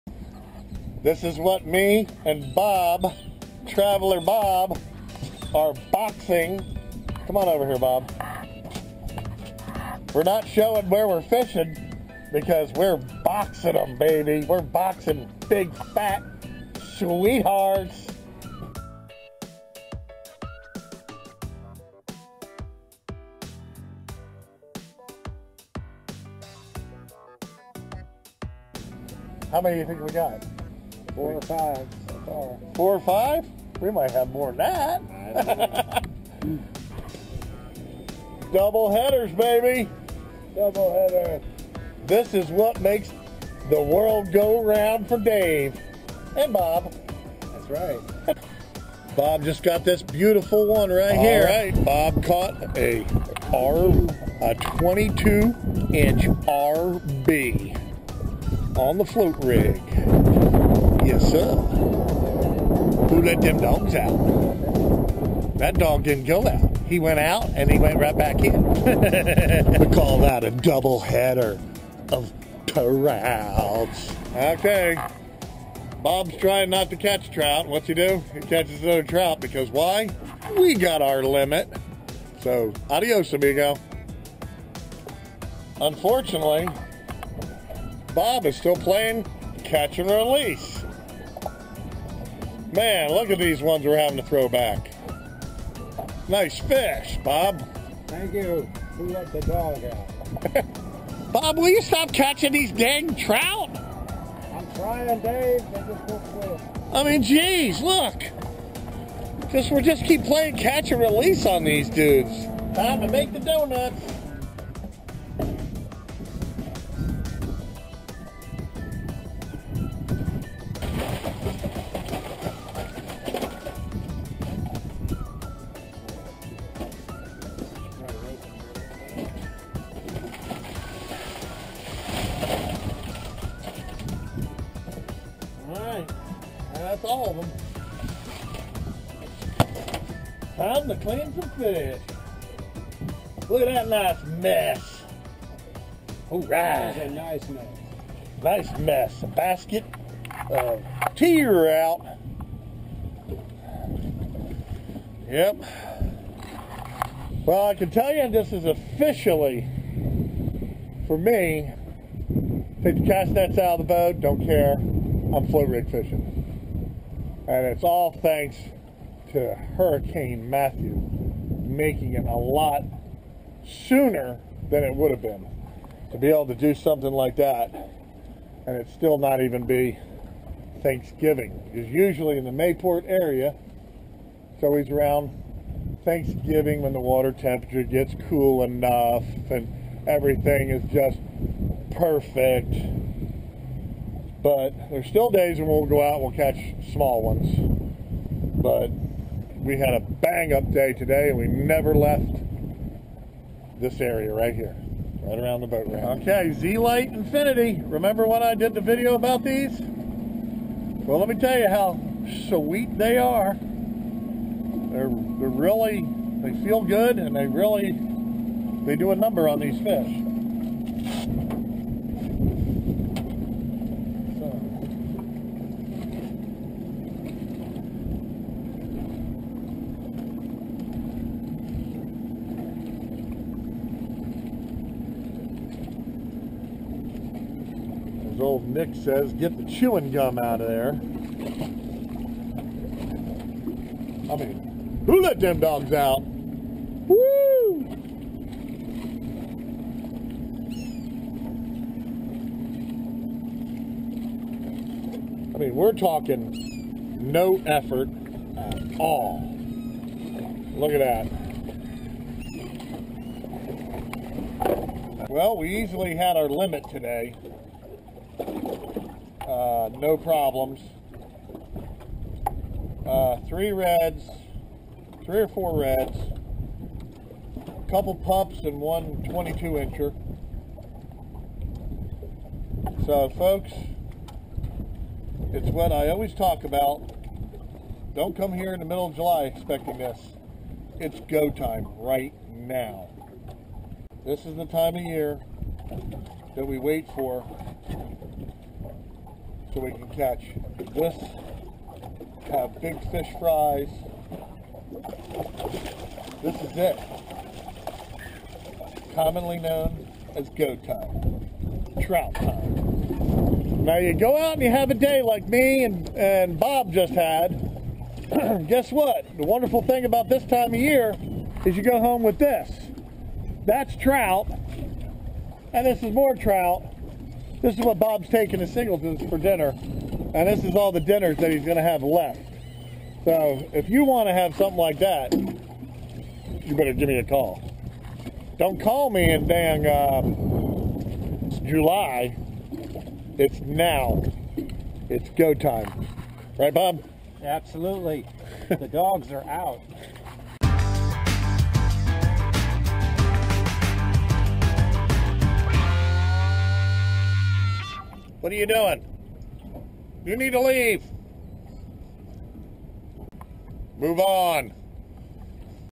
Fishing Report: Oct. Speckled Trout Jacksonville Fl.
"JETTYWOLF LIVE" - great day with long time customer Traveller Bob, whackin & stackin beautiful  Speckled Trout, in Jacksonville Florida on 10/26/2016. HEFTY TROUT, limits, plus Redfish, in the St. Johns River, in the Mayport area. NO LONG BOAT RIDES!!!

it's GO-TIME, reserve your day now!

Float-rig fishing, popping corks, pitching jigs and shrimp is on...from Trout to Sheepshead, book your day N-O-W!
 BIG FISH FRY!